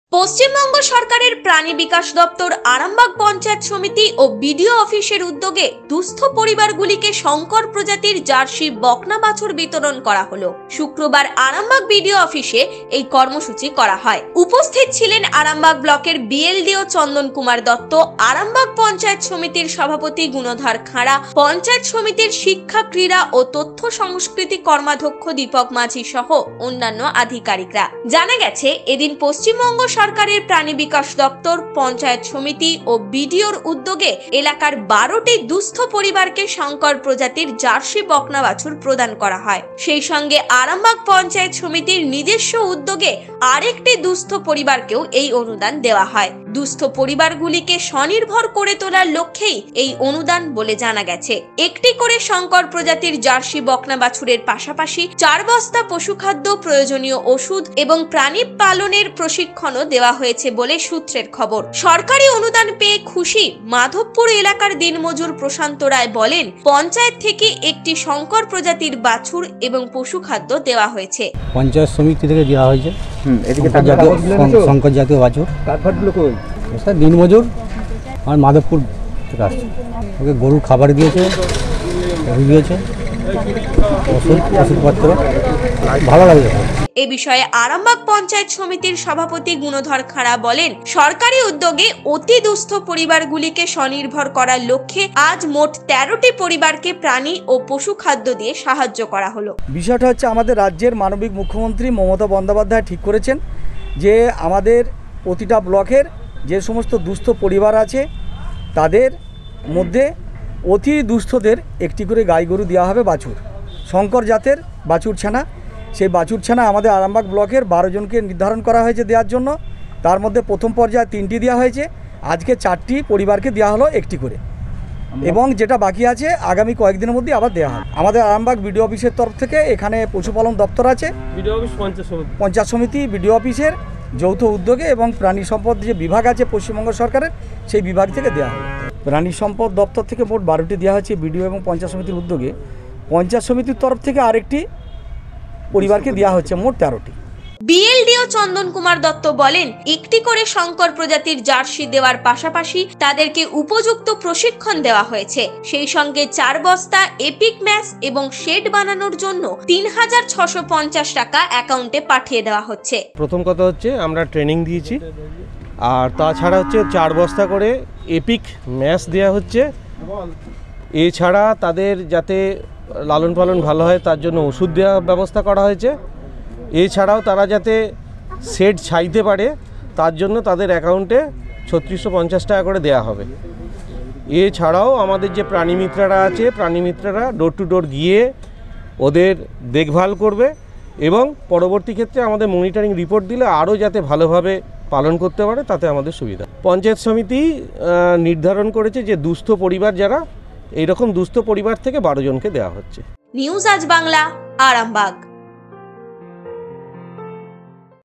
প্রাণী বিকাশ দফতরের বাছুর বিতরণ
পশ্চিমবঙ্গ সরকারের প্রাণী বিকাশ দপ্তর, আরামবাগ পঞ্চায়েত সমিতি ও বিডিওর উদ্যোগে দুস্থ পরিবারগুলিকে বাছুর ও পশুখাদ্য দেওয়া হল।